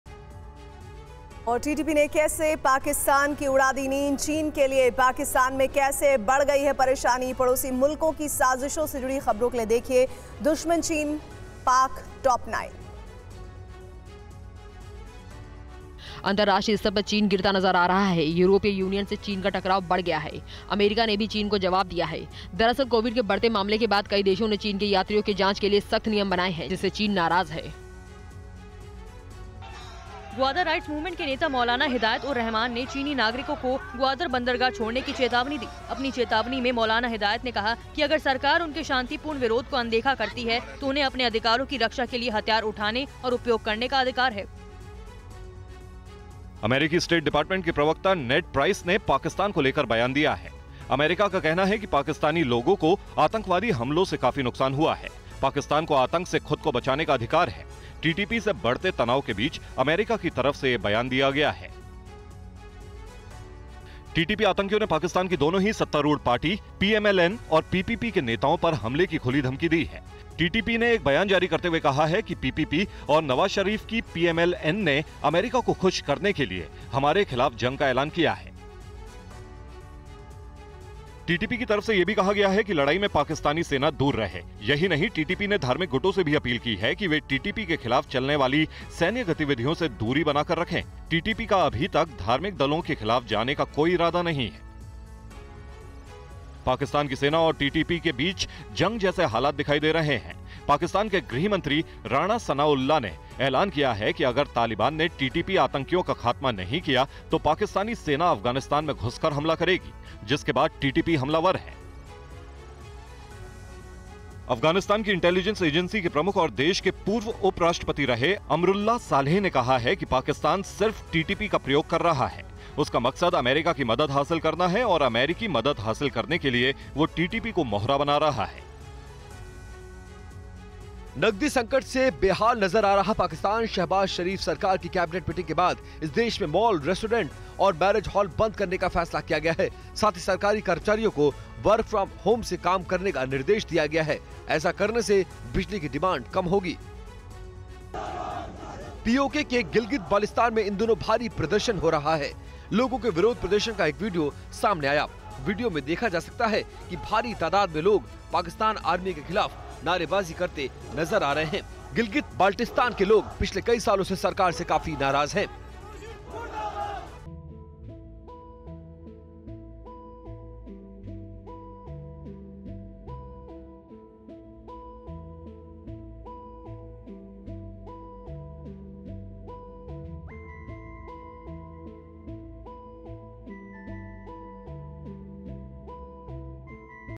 अंतरराष्ट्रीय स्तर पर चीन घिरता नजर आ रहा है | EU | China | Jinping
अंतरराष्ट्रीय स्तर पर चीन घिरता नजर आ रहा है...यूरोपीय यूनियन से चीन का टकराव बढ़ गया है...अमेरिका ने भी चीन का जवाब दिया है...दरअसल कोविड के बढ़ते मामले के बाद कई देशों ने चीन के यात्रियों के जांच के लिए सख्त नियम बनाए हैं...जिससे चीन नाराज है